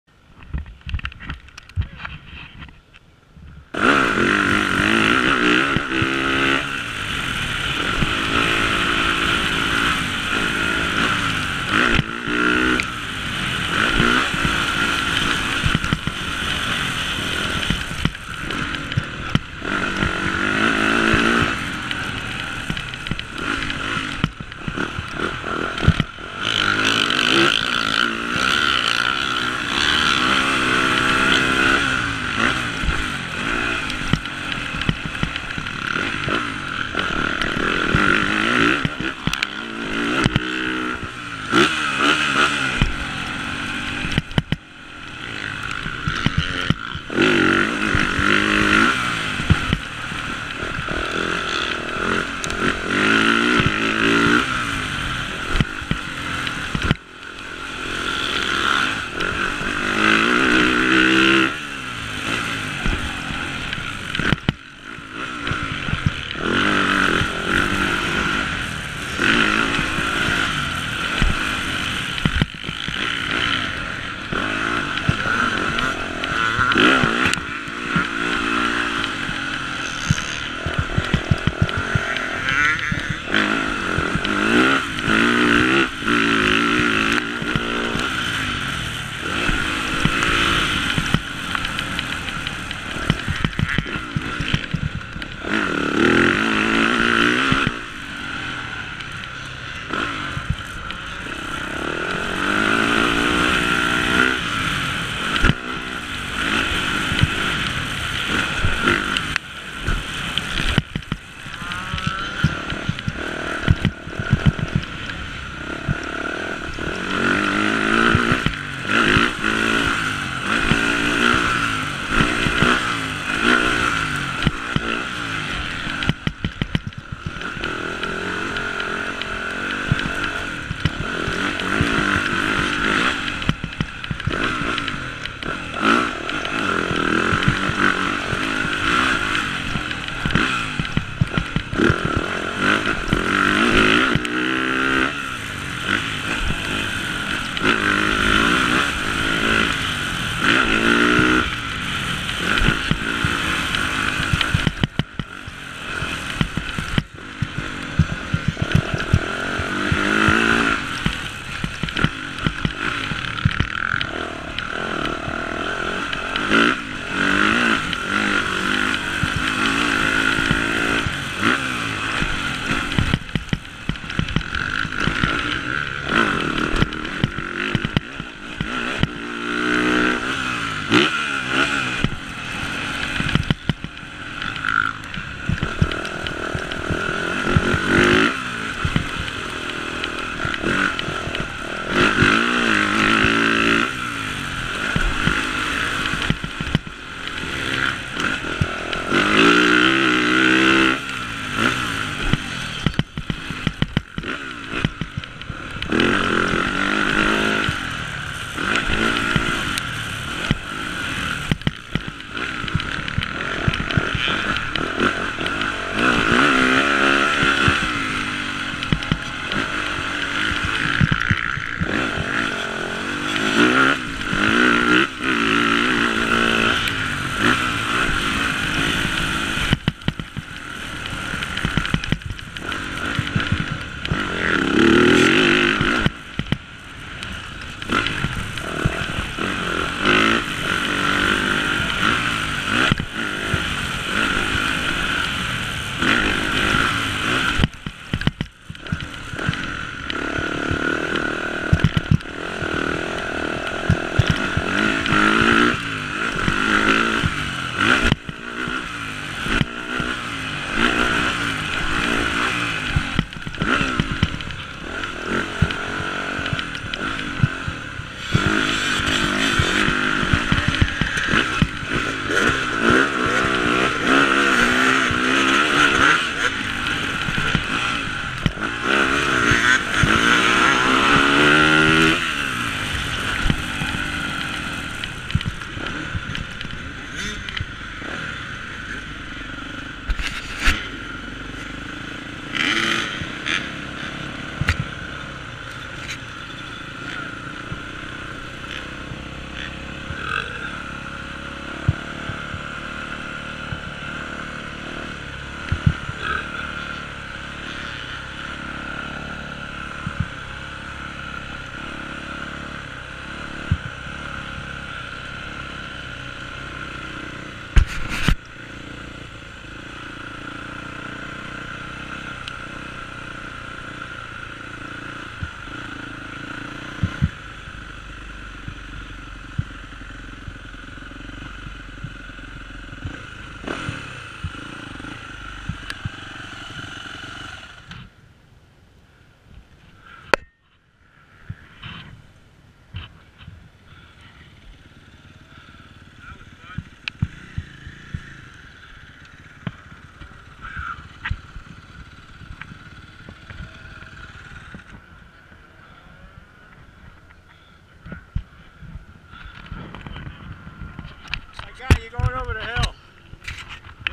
englishtown 10 8 23
wet slick & rutted ,typical etown , b class 3rd moto